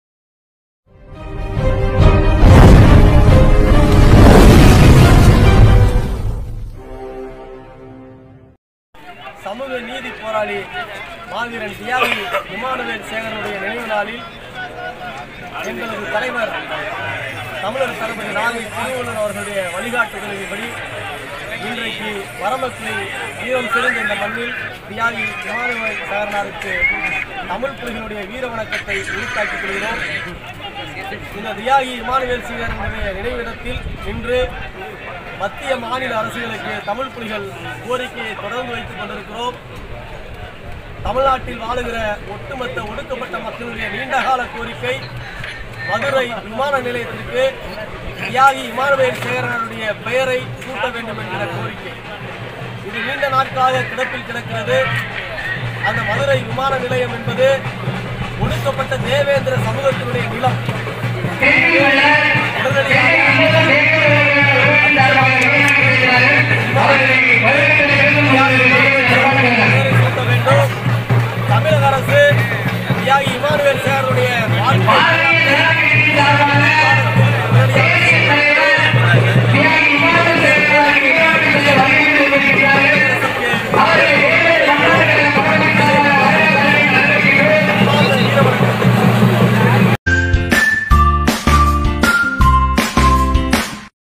மதுரை விமான நிலையத்திற்கு தியாகி இமானுவேல் சேகரனார் பெயர் சூட்ட தமிழ்ப்புலிகள் கோரிக்கை
மதுரை விமான நிலையத்திற்கு தியாகி இமானுவேல் சேகரன் பெயரை சூட்ட வேண்டும் என தமிழ்ப்புலிகள் கட்சி கோரிக்கை